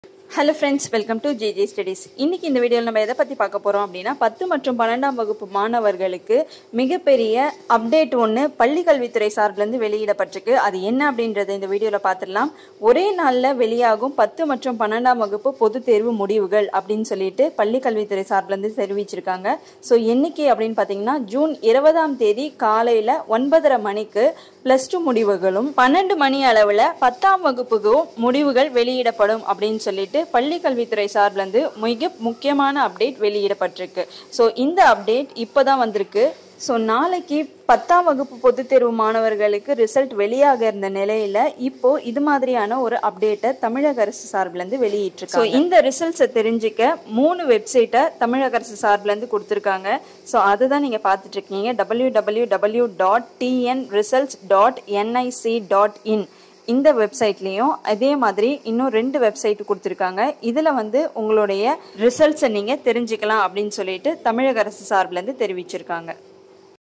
TN 12th & 10th public exam result latest update @gj studies
தமிழக 10 மற்றும் 12ஆம் வகுப்பு பொதுத்தேர்வு முடிவுகளில் திடீர் மாற்றம் பள்ளிக்கல்வித்துறை அதிரடி